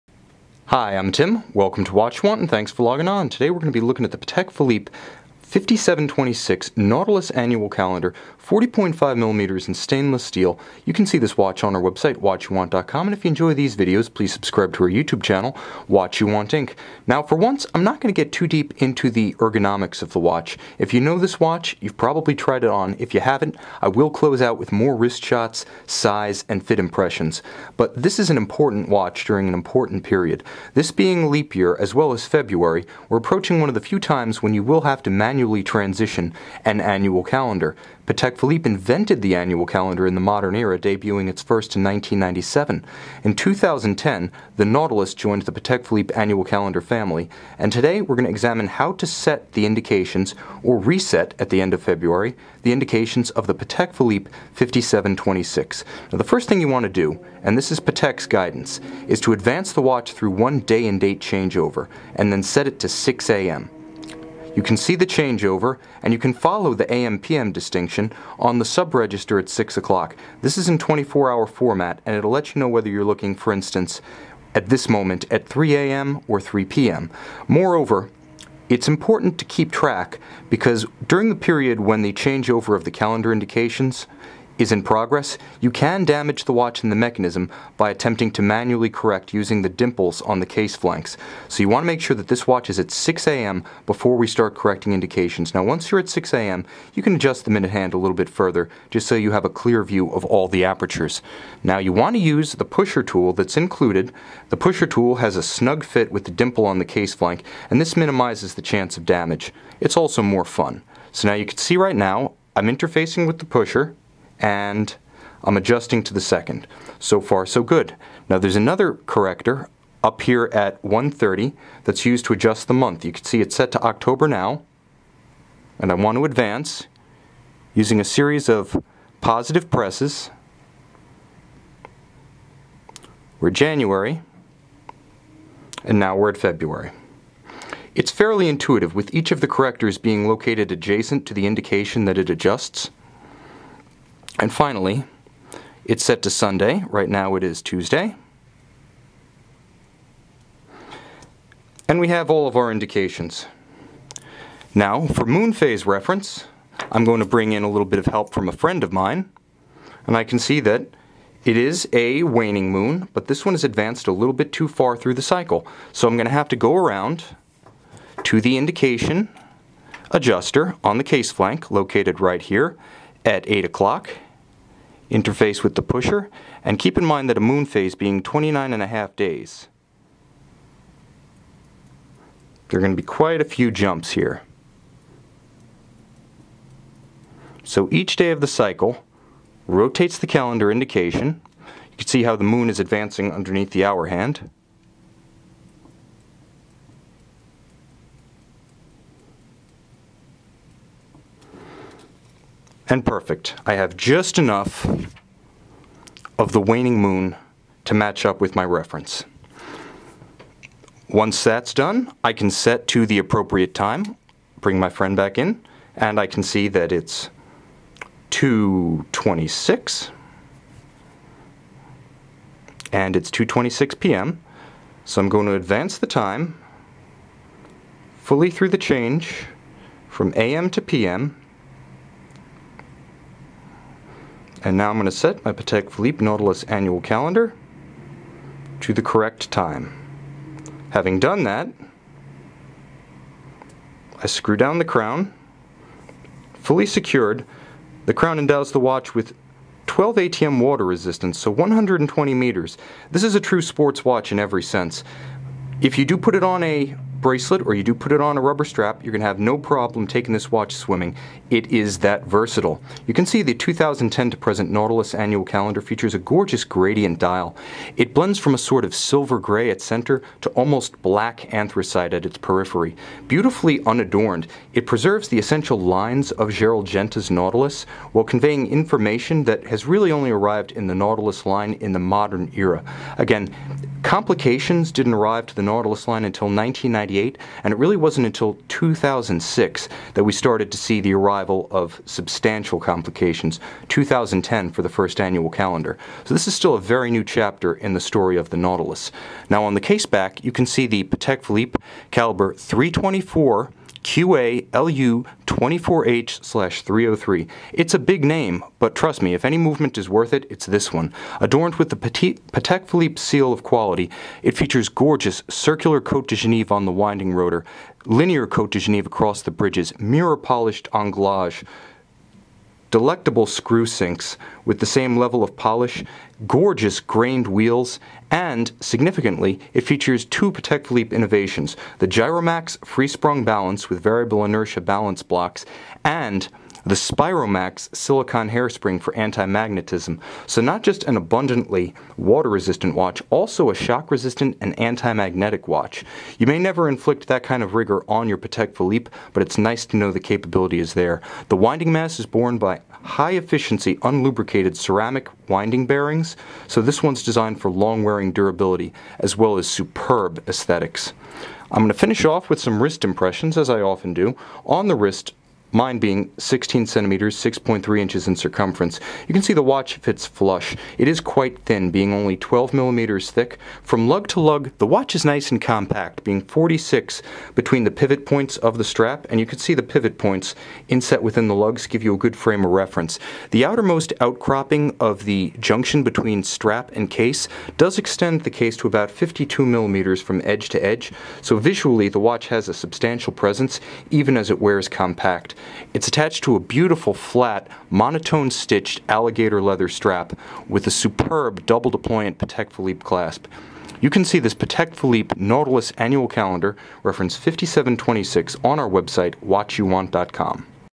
Patek Philippe Nautilus Annual Calendar 5726 Luxury Watch How-To/Review
Watchuwant is now WatchBox!

Watch this video for instructions on how to use the Patek Philippe Nautilus Annual Calendar, reference 5726.

Patek Philippe Nautilus GMT Moonphase (5726A-001) self-winding automatic watch, features a 40.5mm stainless steel case surrounding a grey dial on a brand new black leather strap with a stainless steel deployant buckle. Functions include hours, minutes, day, date, month, GMT and moon phase. This watch comes complete with box, papers and an extra OEM black leather strap.

See this 40.5mm stainless steel Patek Philippe Nautilus Annual Calendar 5726 in high-resolution images on our